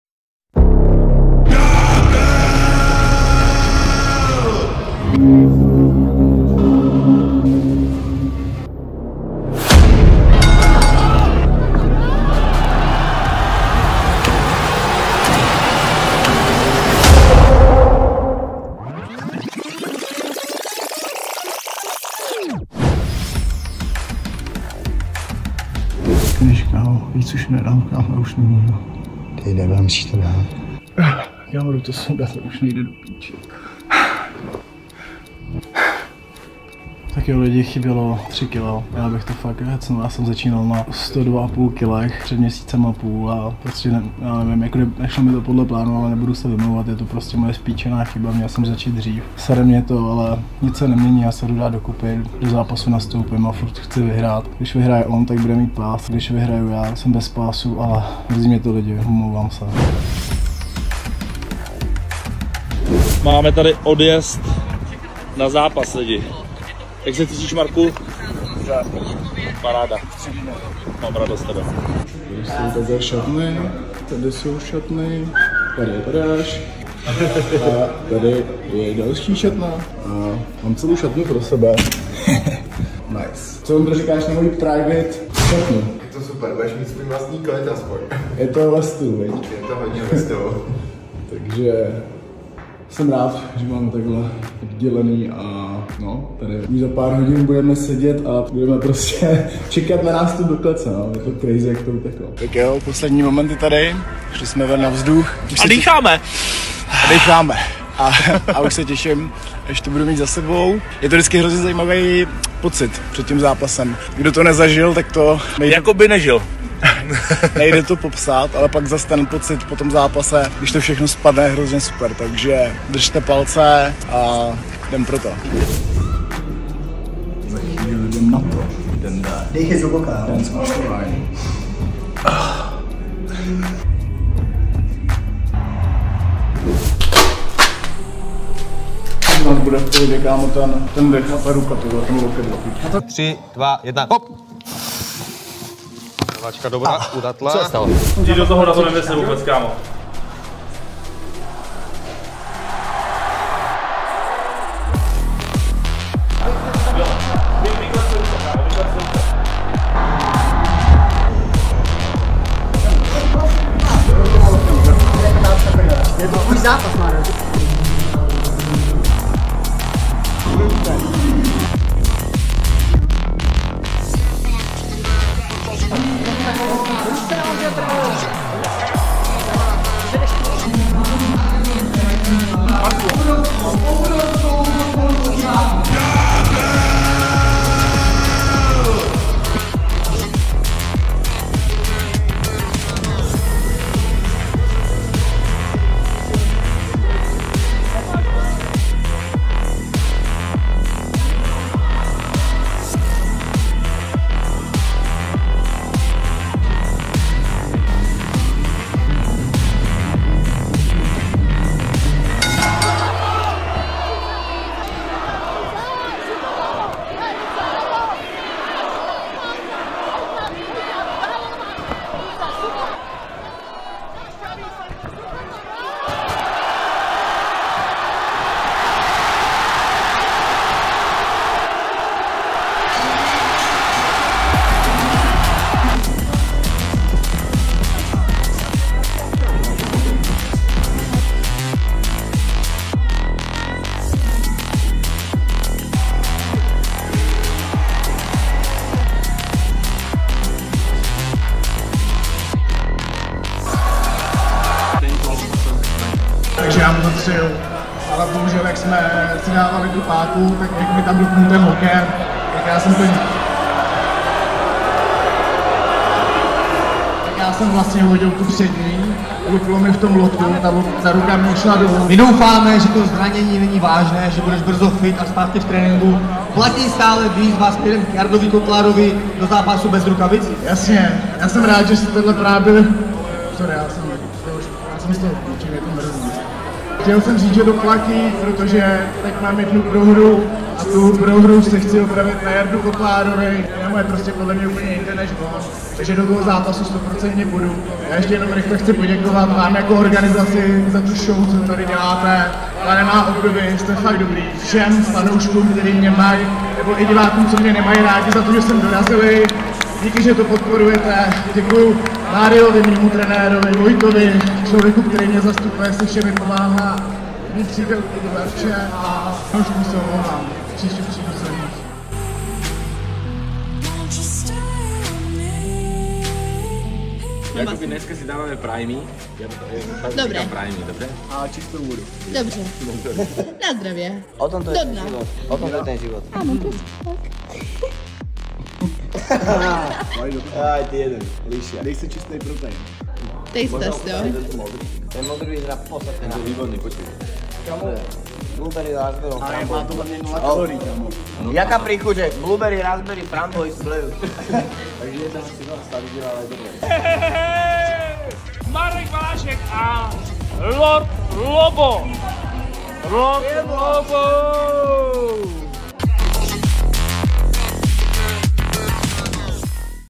Jak jsem prohrál můj nejdůležitější ZÁPAS.. Datel VS DYNAMO FIGHT
V dnešním videu uvidíte můj zápas v Clash of the Stars s Dynamem a na víc uvidíte co se dělo těsně před zápasem a po zápase.